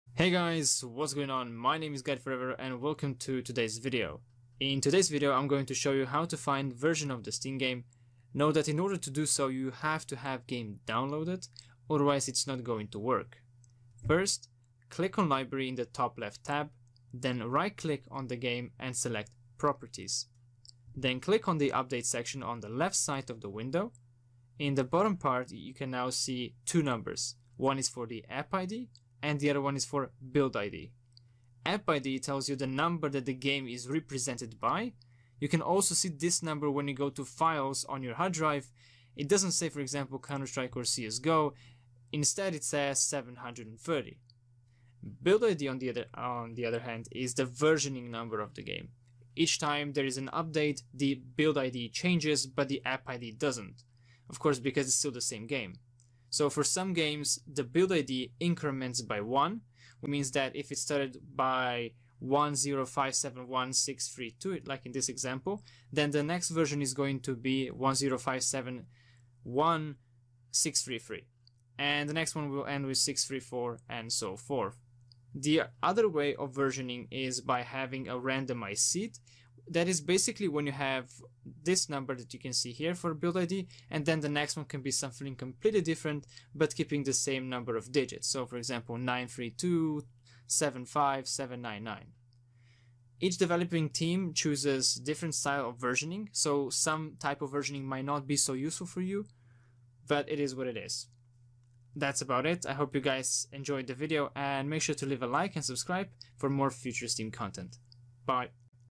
Where to Find Game Version in Steam (2023)
In this tutorial I am going to show you how and where to find game version in Steam. I will also explain the logic behind the versioning and the methods used so as for the viewer to understand such concept.

Transcript:
Hey guys,
what's going on, my name is Guide4Ever and in today's video I am going to
show you how to find version of the Steam game. Note that in order to do so
you have to have the game downloaded otherwise it's not going to work.

First, click on Library in the top left tab, then right click on the game
and select Properties. Then click on Updates section on the left side
of the window. In the bottom part you can now see 2 numbers, one for App ID
and the other one for Build ID. App ID tells you the number that the game
is represented by. You can also see this number when you go to files on
your hard drive, it doesn't say CSGO for example but it says 730 instead.
Build ID is the versioning number of the game. Each time there is an update
the Build ID changes, for some games it increments by 1, which means
if it started with 10571632 then the next version is going to be 10571633,
the next one will end ...634 and so forth. The other way of versioning is by
having a randomized seed, that is basically when you have 10571632
for example, but the next number can be something like 93275799, in other
words something completely different.
Each developing team chooses different style of versioning their game
so it might not be useful sometimes especially if the version is randomized
each time they make an update.

That's about it,I hope you guys enjoyed the video and make sure to leave